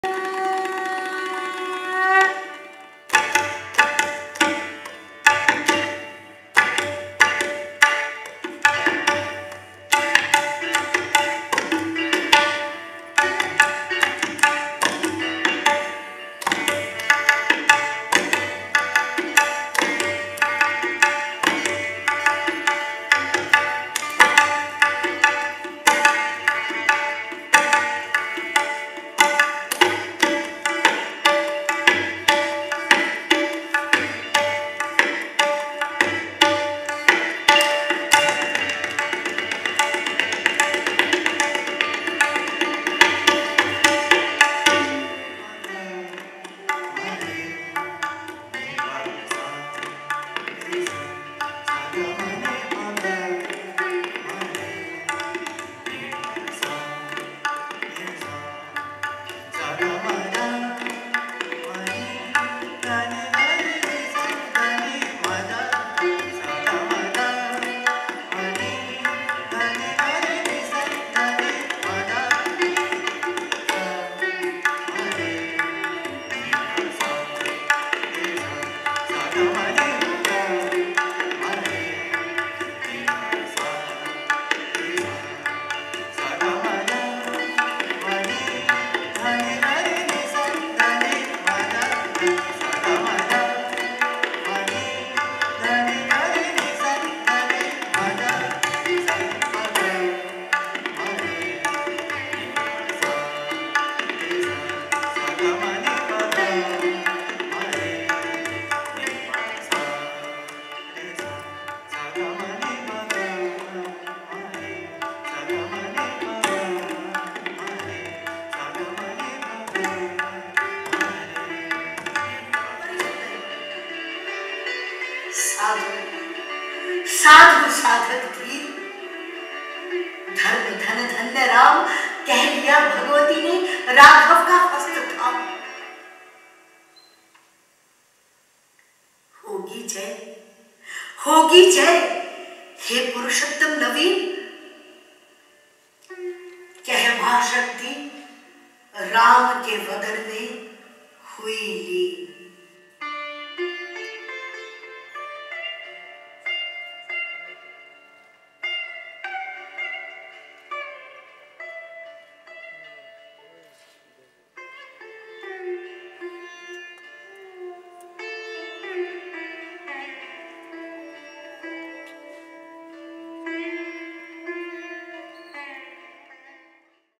AZADI KI LEHER 2 & SHRADDHANJALI II SASWATI SEN & VASWATI MISRA II DUET II ABHINAYA
Nrityangan Kathak Kendra organises Azadi Ki Leher 2 & Shraddhanjali at EZCC Kolkata on 13th & 14th August, 2023. This festival was graced by Padmavibhushan Dr. Sonal Mansigh, Vidushi Saswati Sen, Shri Ratikant Mohapatra Vidushi Vaswati Misra and many more.